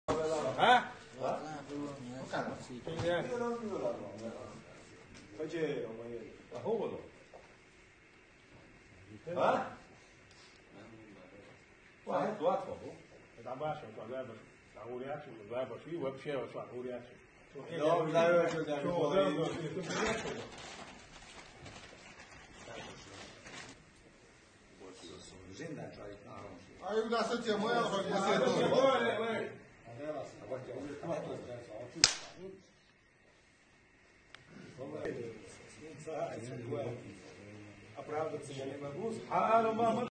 Абу Шейх Висмурадов ударил чеченского старика
Вот настоящее лицо кадыровского режима, подобное происходит сегодня во всех отделах Чечни каждый день.

Путь Ахмата такой - какой он есть